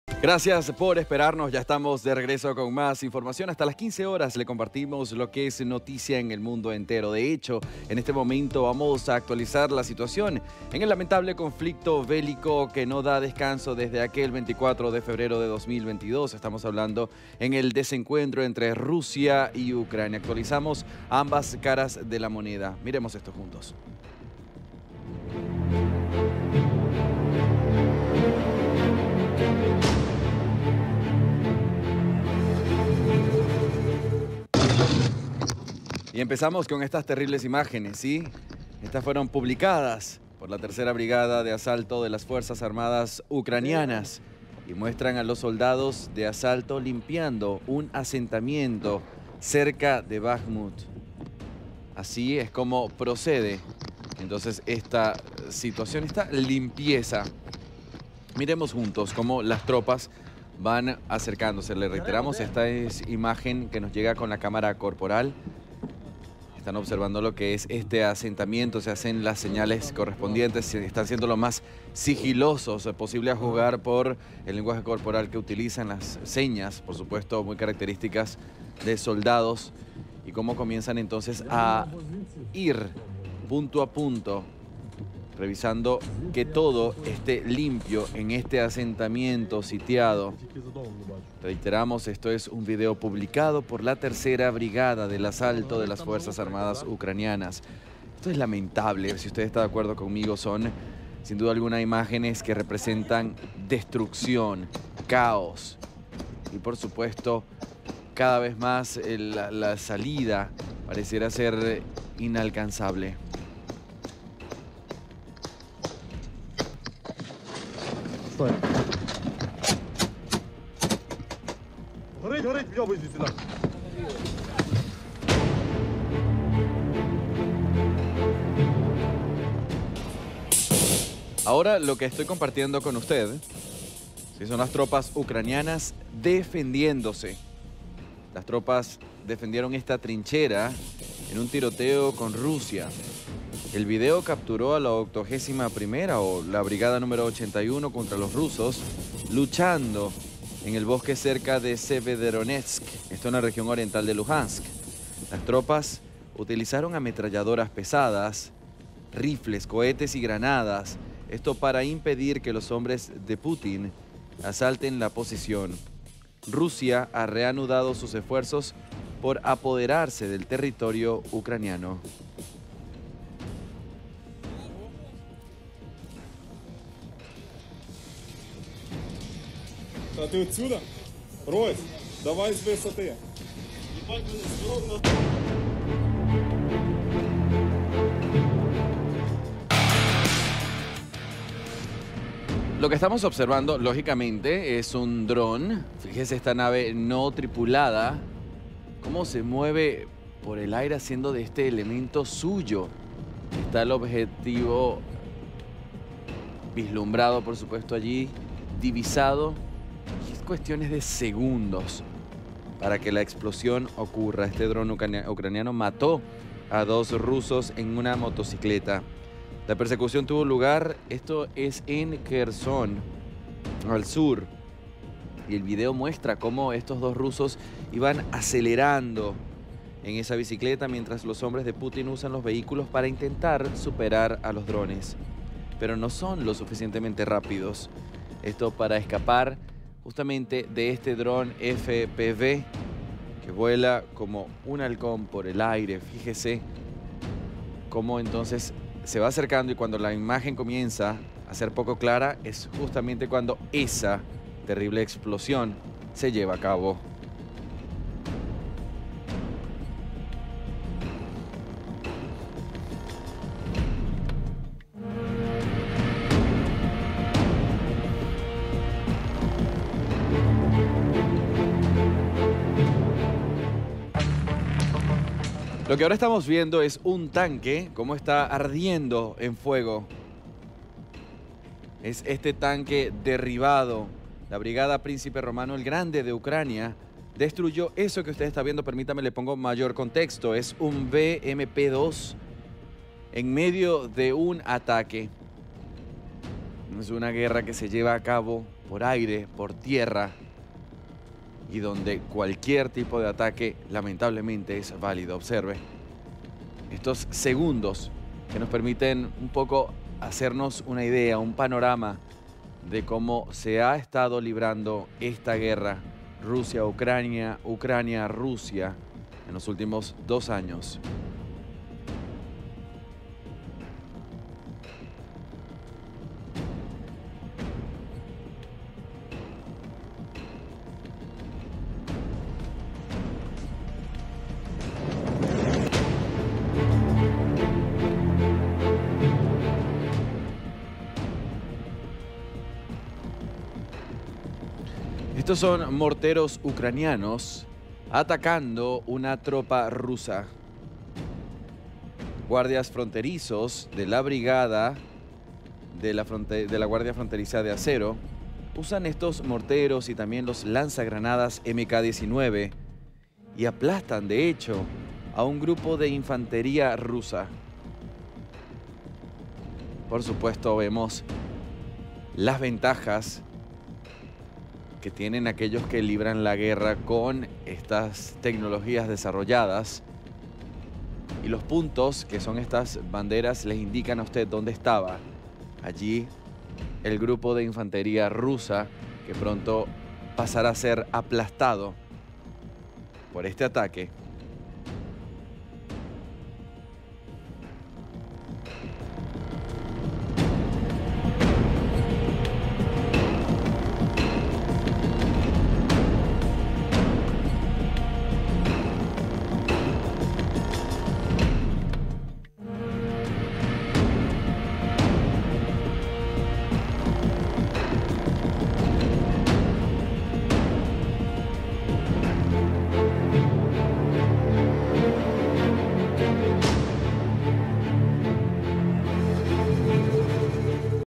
GUERRA RUSIA-UCRANIA: Así se defienden las tropas ucranianas en una trinchera en medio de un combate
👉 Las tropas ucranianas defendieron su trinchera en un tiroteo con Rusia. El vídeo capturó a la 81.ª Brigada luchando contra los rusos en el bosque cerca de Severodonetsk, en la región oriental de Lugansk.